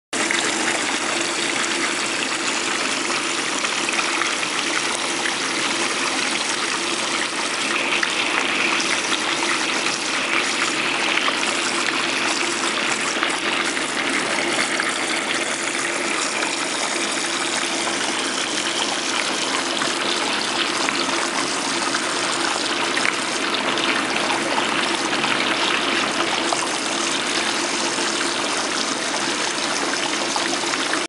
River Current with Superbubbles in the TreeFrog by Multiponics
Shows the TreeFrog by Multiponics configured for River Current with SuperBubbles, aka under current or current culture. This setup is running about 600GPH through the 4' x 4' system.